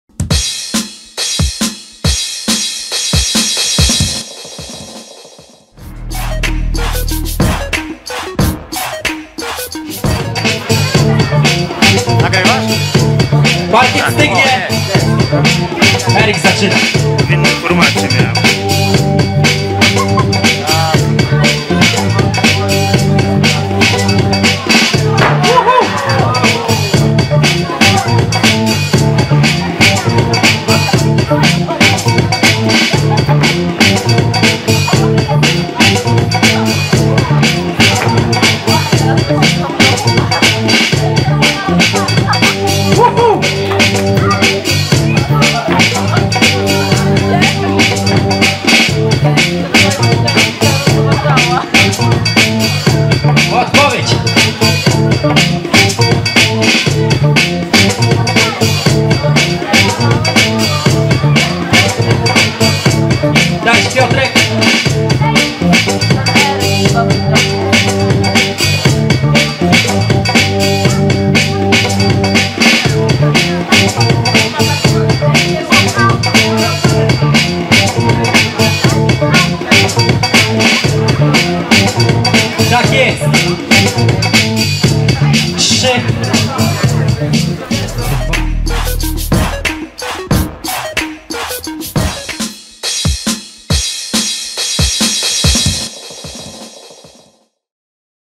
13th Anniversary OCB / Półfinał Bboying 1vs1 do 14 lat / Ministrant vs Eryk
13th Anniversary OCB / Półfinał Bboying 1vs1 do 14 lat / Ministrant vs Eryk / Białystok - Dziesięciny / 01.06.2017.